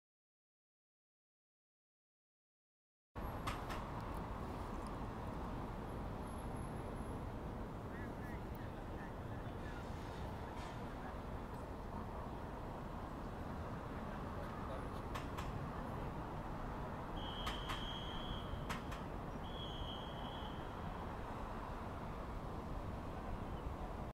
21 seconds: Gate With History
Filmed in Washington, DC. This video of function shows the Home Plate Gate outside Nationals Park with the history of DC Baseball. This video if provided free to use under Creative Commons-Attribution license.